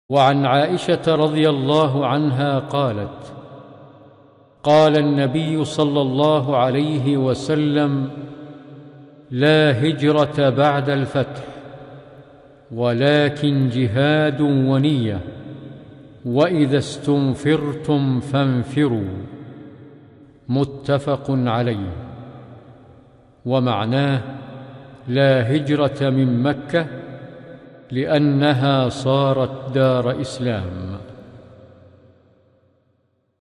حديث 3 رياض الصالحين - " لا هجرة بعد الفتح، ولكن جهاد ونية، وإذا استنفرتم فانفروا"
3- وعن عائشة رضي الله عنها قالت : قال النبي صلى الله عليه وسلم :

" لا هجرة بعد الفتح، ولكن جهاد ونية، وإذا استنفرتم فانفروا" متفق عليه.

ومعناه: لا هجرة من مكة لأنها صارت دار إسلام.

ستجد في القناة  أيضا  القرآن الكريم بصفحات المصحف الشريف